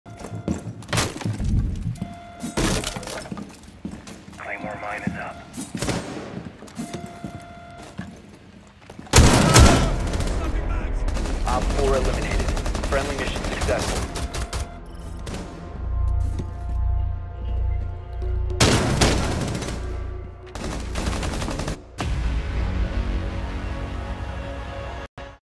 I Feel Bad For This Recruit (Read Description Before Watching)
I felt so bad for the recruit. We had 2 consecutive wins and only needed one more. The whole other team left besides the recruit. So instead of the match being how to kill the enemy effectively, it became a match of who could rush them with shotgun first. I won.